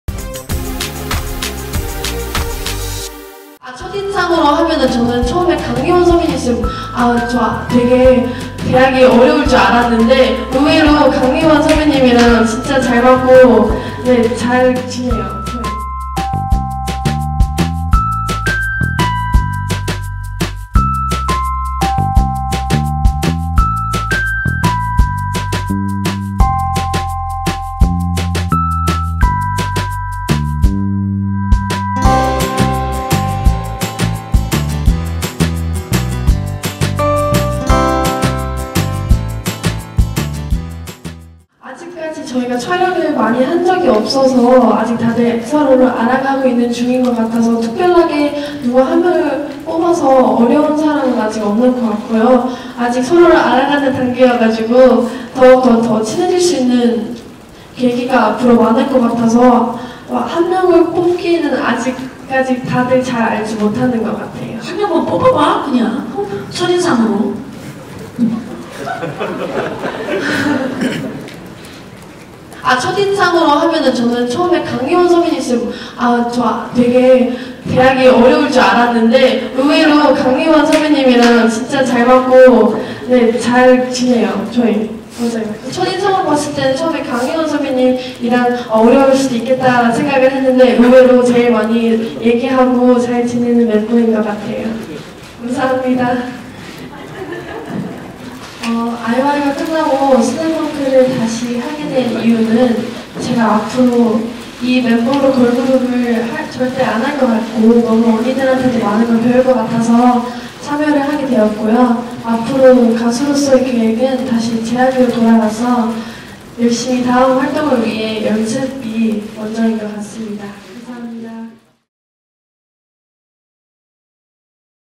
[TV10] 전소미 "멤버들 첫인상? 강예원 대하기 어려웠다" (KBS2 '언니들의 슬램덩크 시즌2')
[TV10] 2월 8일 KBS2 '언니들의 슬램덩크 시즌2' 제작발표회 현장